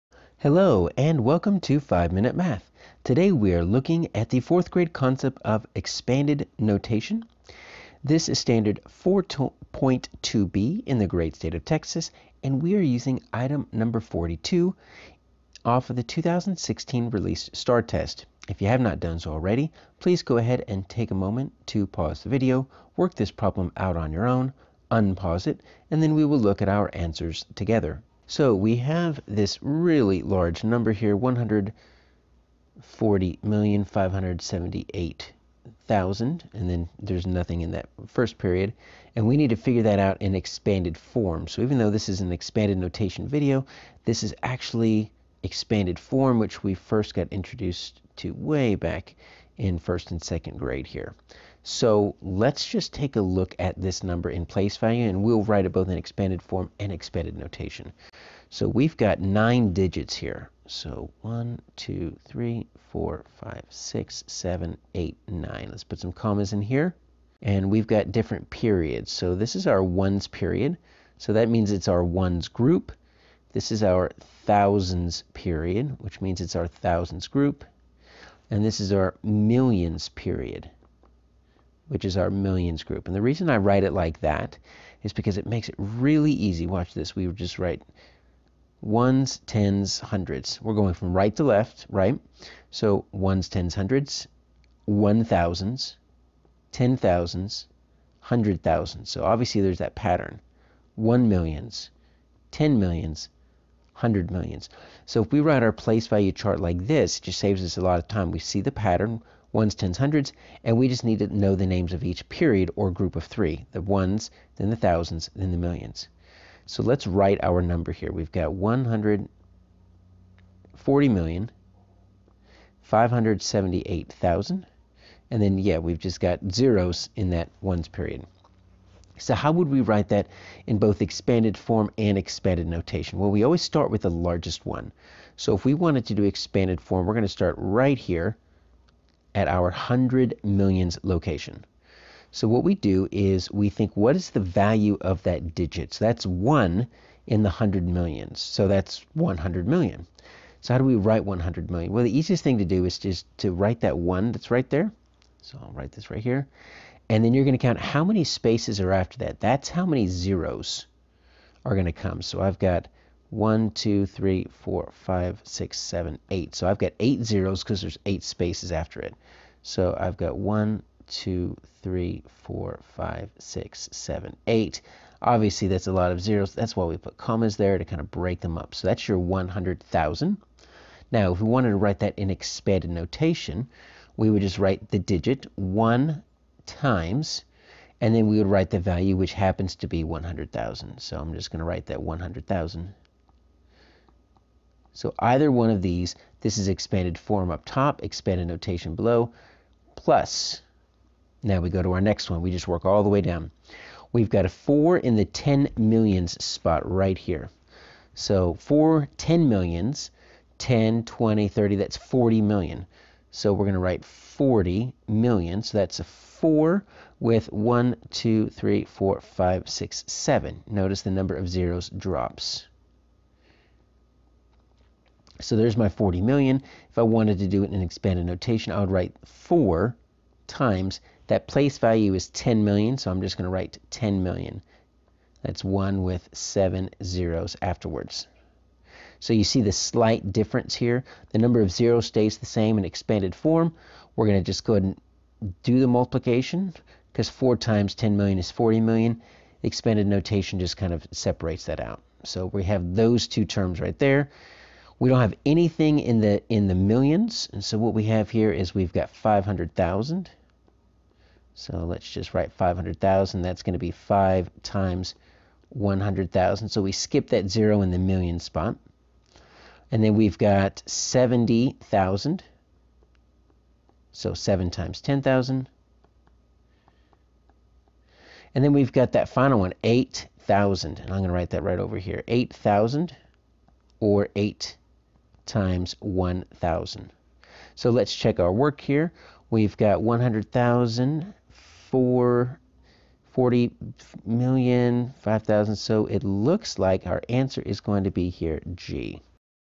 4th Grade STAAR Practice Expanded Notation (4.2B - #2)
Practice representing the value of the digit in whole numbers through 1,000,000,000 and decimals to the hundredths using expanded notation and numerals with a test item from the 2016 released STAAR test (#42 - 4.2B).